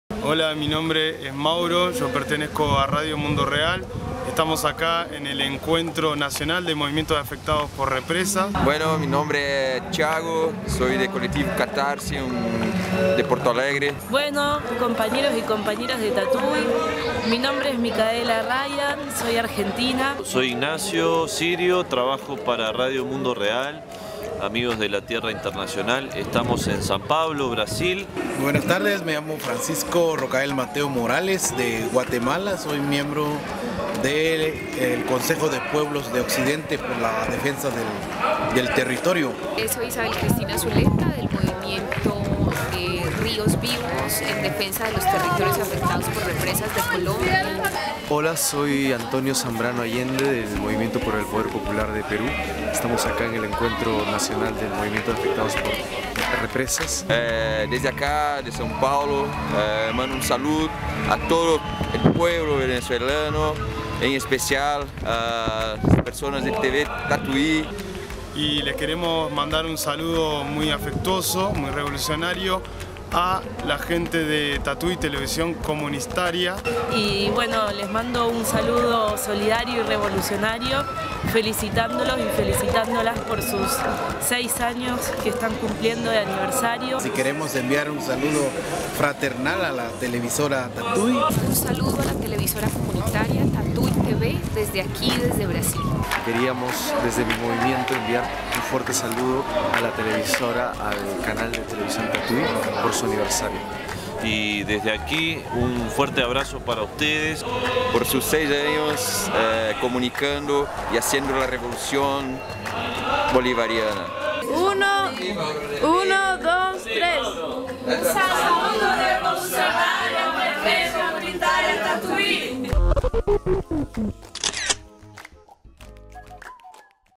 Saludos Nuestro Americanos para Tatuy Tv
Tatuy Tv, A través de la red ALBA Tv, Participó en el encuentro nacional de afectados por represas, desarrollado en Sao Paulo, Brasil, durante el mes de Septiembre de 2013, desde allí, compañeros y compañeras de distintas regiones del continente envián su afectuoso saludo por nuestro Sexto Aniversario, en lucha franca contra la hegemonía mediática y por la construcción de una nueva subjetividad socialista.

Tatuy Tv, Afinando el Tino, es decir, la puntería.